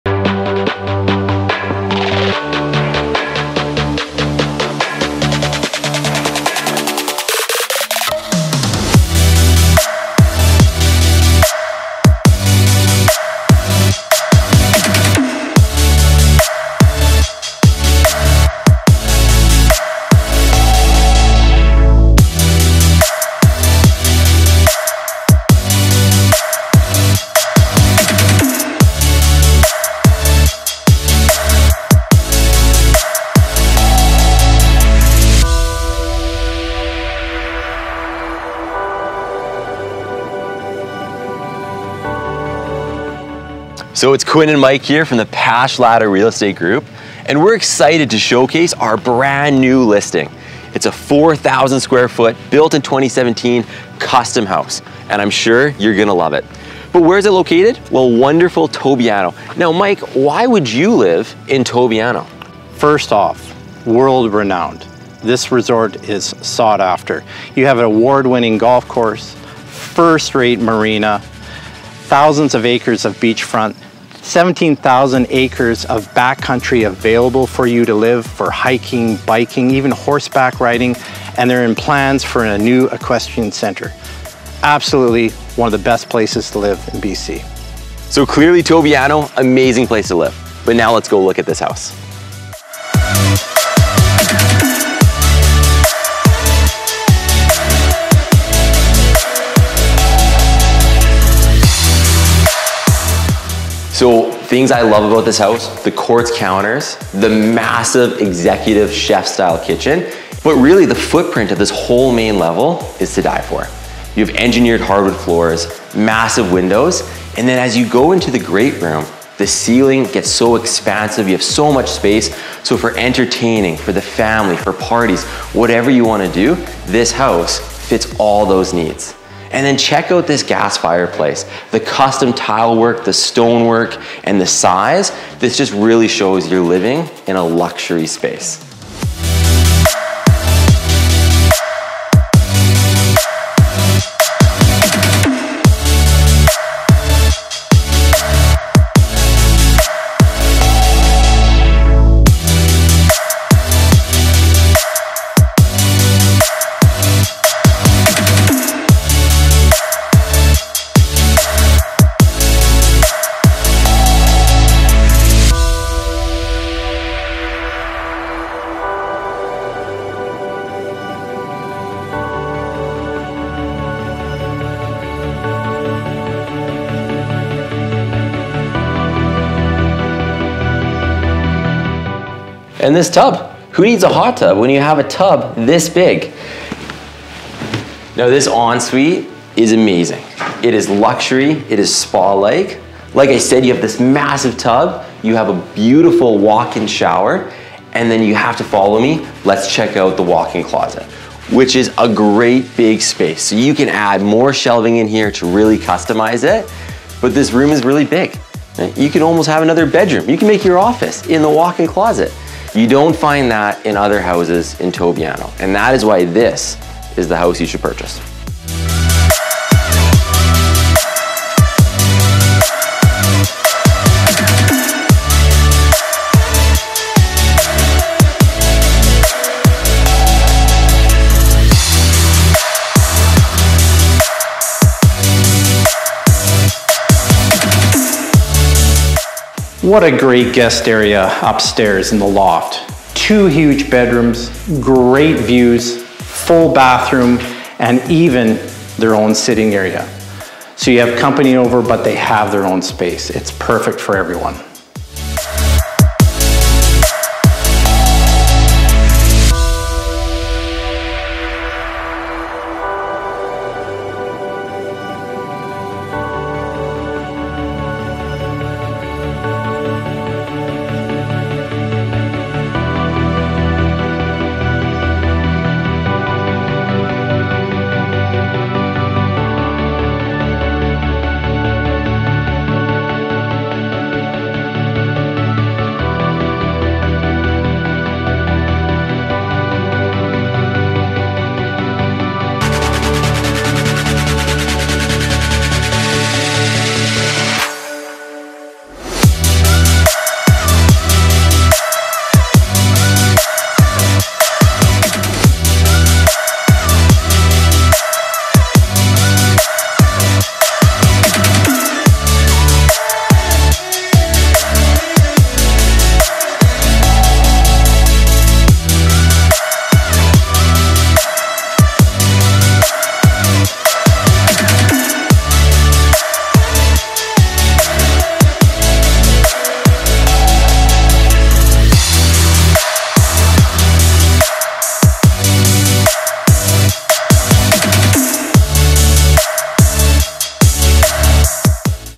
213 Rue Cheval Noir • Luxury Home Real Estate Video in Tobiano BC
Welcome to 213 Rue Cheval Noir located in Tobiano- an exquisite custom-built approx 5000 sqft home. With 6 beds and 4 baths, this modern home is designed with an executive custom kitchen.The great room is an ideal entertaining space with floor to ceiling windows, exposed fir beams, hardwood floors, fir & metal accents & a beautiful large tiled fireplace. Mountain views from the patio provides a relaxing space to wind down. This home is fully landscaped with fruit trees & a mature garden!The view from master suite on the main floor makes it feel like an oasis with its own private view, 5 pc ensuite with large jetted soaker tub & XL walk in closest. Upstairs provides 2 bedrooms, a 4 pce bath, private sitting area & sunrise/sunset, lake & mountain views. Downstairs has a large rec room, bedroom and a family in-law suite. Parking is ample with a 3 car garage that is close to 1000 sq ft and has an EV charger. New Central A/C.
Listing by Mike Latta *Personal Real Estate Corporation - Partner of the Pache Latta Real Estate Group